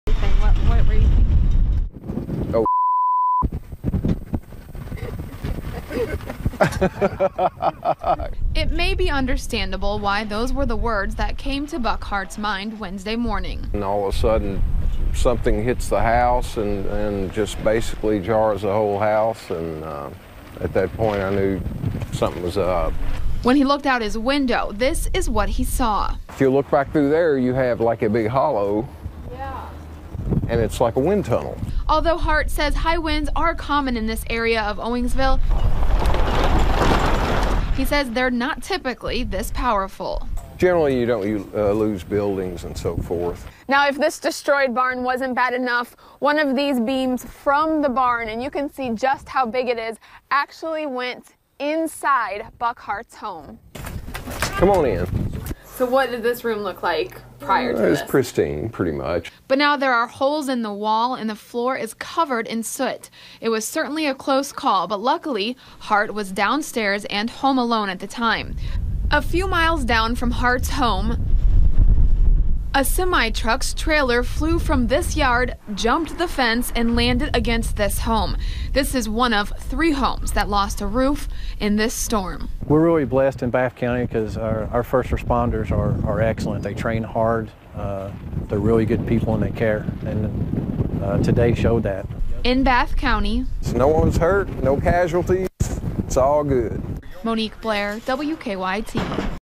Owingsville, Ky storms (Bath County)
Storms swept through Kentucky on March 1, 2017.